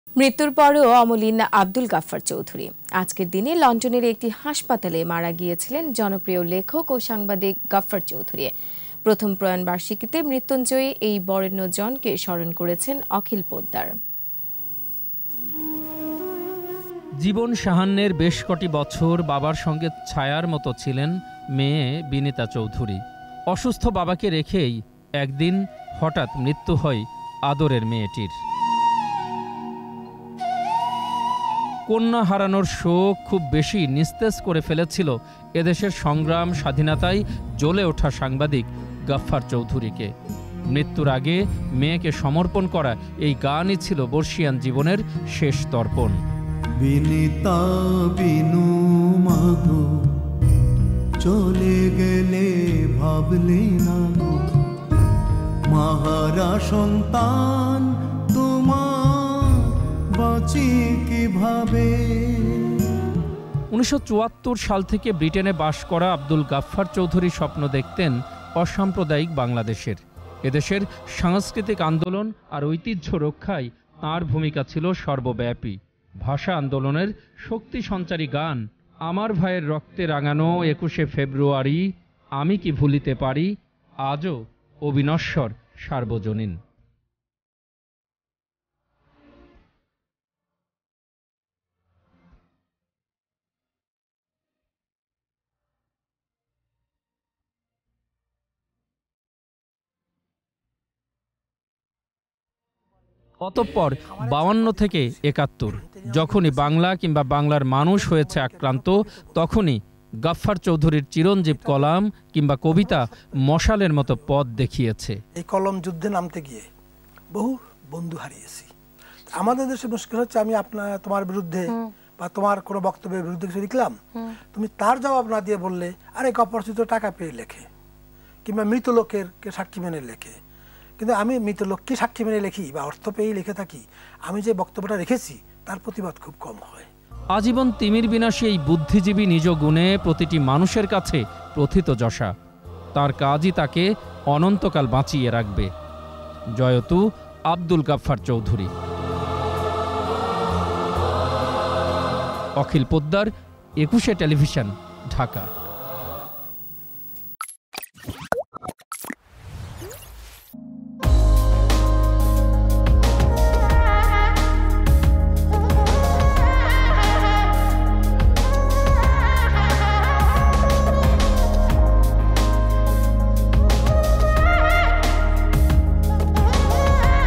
মৃত্যুর পরও অমলিন আবদুল গাফফার চৌধুরী || ETV News
মৃত্যুর পরও অমলিন আবদুল গাফফার চৌধুরী। আজকের দিনে লন্ডনের একটি হাসপাতালে মারা গিয়েছিলেন জনপ্রিয় লেখক ও সাংবাদিক গাফফার চৌধুরী। প্রথম প্রয়াণ দিবসে মৃত্যুঞ্জয়ী এই বরেণ্যজনকে স্মরণ করেছেন অখিল পোদ্দার।